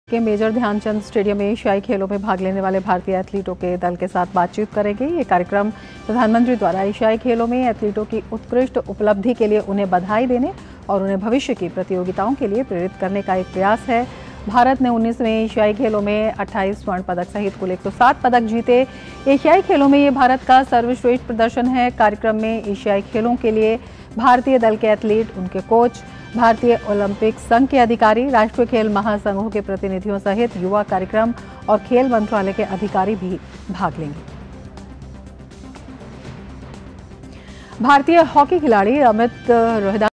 19वें एशियाई खेलों के भारतीय दल से संवाद करेंगे पीएम मोदी
19वें एशियाई खेलों के भारतीय दल से संवाद करेंगे पीएम मोदी